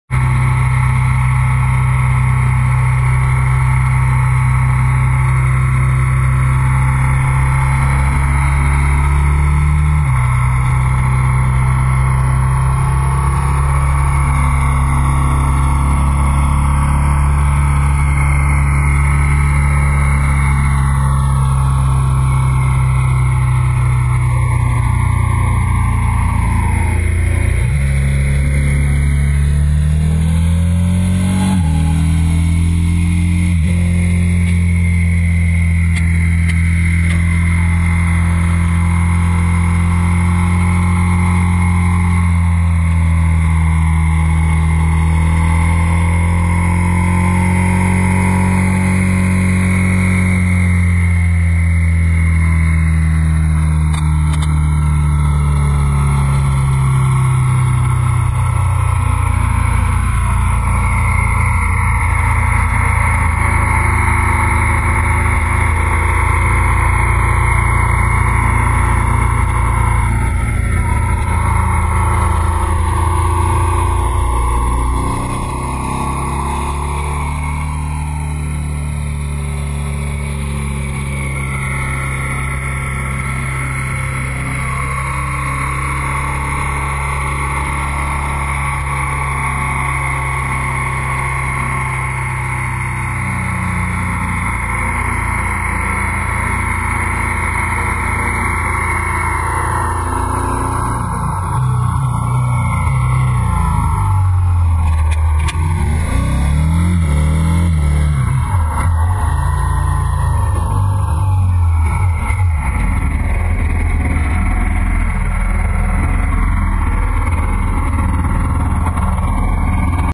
POV CAM Fast Motorcycle Ride Dual View 04012012-1
A 2 camera POV of a motorcycle ride down a two lane country road